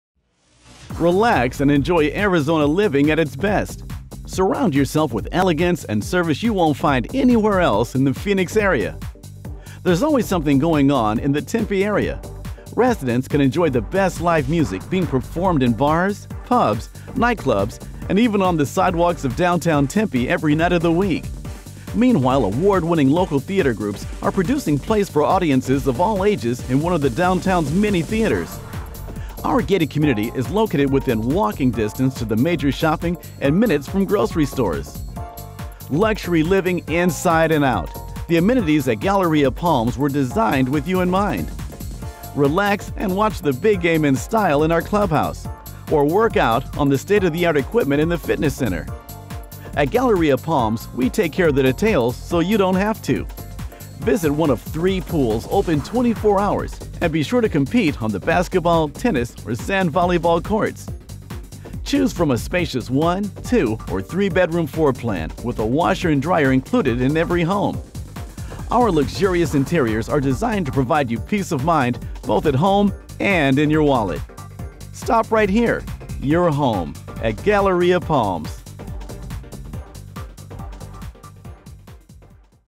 ForRent.com Galleria Palms Apartments in Tempe, AZ
Relax and enjoy Arizona living at its best. To live at Galleria Palms is to surround yourself with elegance and service you won't find anywhere else in the Phoenix Area. Luxury Living Inside & Out The amenities at Galleria Palms were designed with you in mind. Relax and watch the big game in-style or work out on the latest elliptical trainer in the fitness center. At Galleria Palms, we take care of the details so you don't have to. Minutes from downtown & Sky Harbor International Airport Friendly, Proffesional Staff Private, Gated Community Assigned, Covered Parking Private Garages available Full-size washer/dryer in every apartment home Additional private storage available High-speed Internet Access available Amazing Indoor Basketball Court State-of-the-art Fitness Center Indoor Movie Theater Resident Business Center Tennis & Sand Volleyball Courts Three Sparkling Swimming Pools & Two Soothing Spas Galleria Palms Apartments offers an array of services to meet the needs of our valued residents. At Galleria Palms, your comfort and quality is our principal concern. Friendly management staff available 7 days a week. Professional engineering and janitorial crew. Courtesy patrol services. After hours emergency maintenance available. Expanded cable service available. Business Center with high speed Internet access. Fax and copier service. Package retrieval. Galleria Palms Apartments' central location is close to many great restaurants, theaters, nightclubs, pubs and all types of family entertainment. Considered one of the hottest spots in the Valley of the Sun, downtown Tempe has long been known as the entertainment center of the Phoenix Metro area. There's always something going on in the Tempe area. Residents can enjoy the best live music being performed in bars, pubs, nightclubs and even on the sidewalks of downtown Tempe every night of the week. Meanwhile award winning local theater groups are producing plays for audiences of all ages in one of the downtown's many theaters.